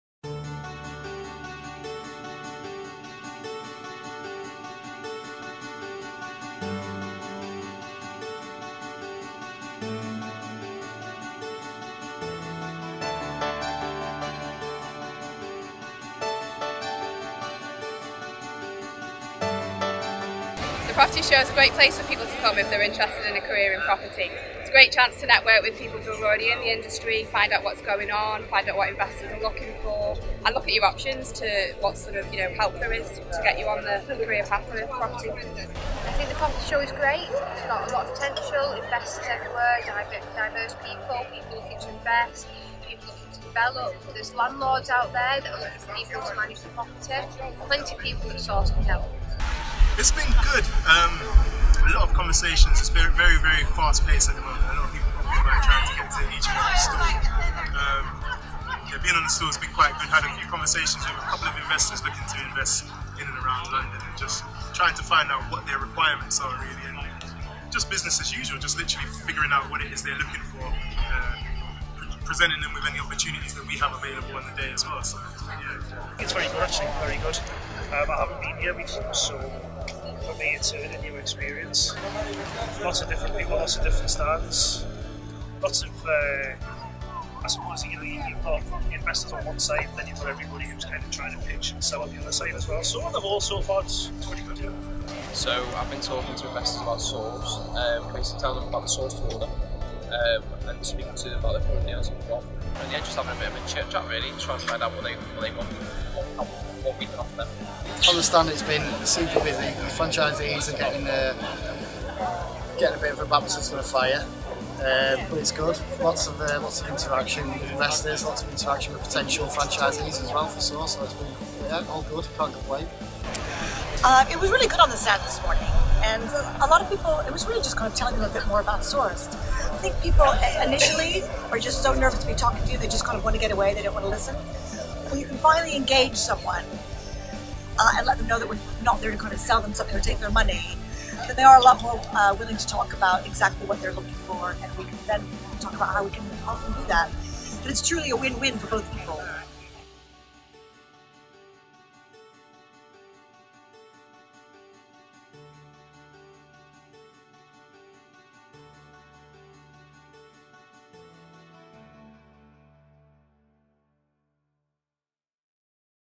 Property Investors Show - April 2018
Check out what we thought about the Property Investors and Homebuyers Show in April 2018 and how we can help investors to invest in property.